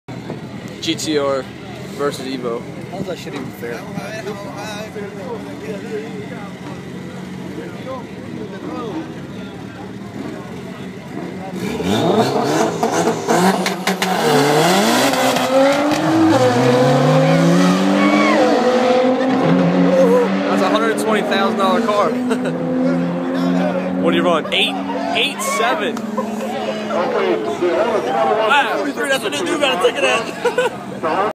Gtr vs Evo
Import face off at Capitol raceway in Crofton, MD . First run of the day for the GTR and he ran a 8.7 .